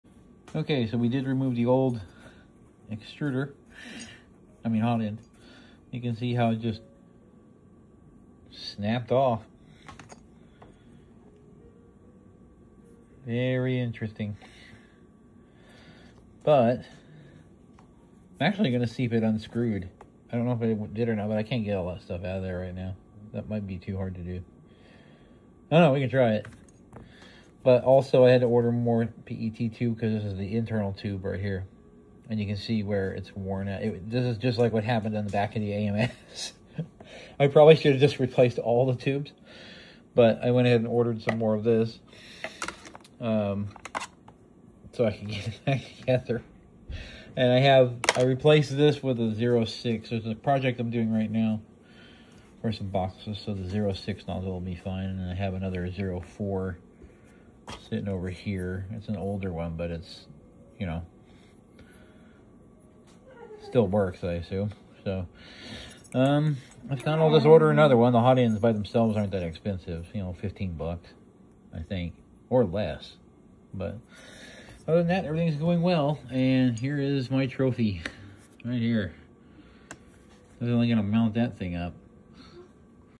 My printer is begging for mercy. 🤣
There were a few smaller issues that had been overlooked on the inside of the printer. I’m wearing out filament tubes. I am definitely giving this printer a work out. Talk about a 3-D printer, torture test.